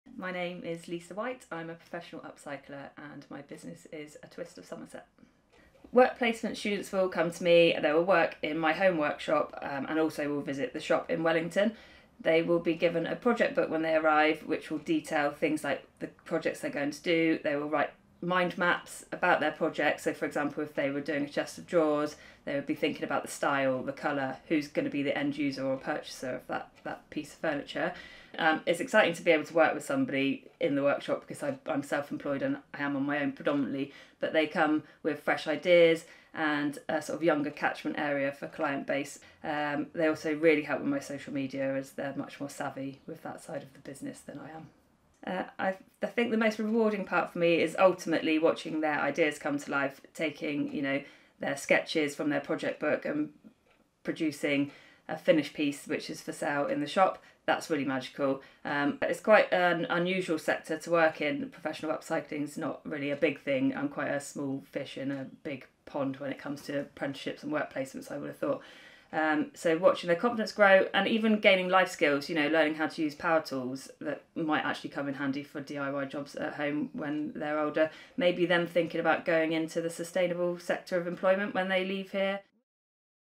Employer Interview-A Twist of Somerset and BTC: Self-Employed Excellence
The partnership between A Twist of Somerset and Bridgwater and Taunton College (BTC) is a tribute to woodworking craftsmanship, custom tables, and the cultivation of emerging carpentry talent. Discover how this collaboration is not only shaping the future of wood artisans but also how it can empower your organization and celebrate the artistry of Somerset's wood creations.
Through the A Twist of Somerset and BTC partnership, we become part of a network of woodworking professionals and organizations. This network keeps us informed about the latest woodworking trends and innovations, fostering creativity and excellence in our woodcraft.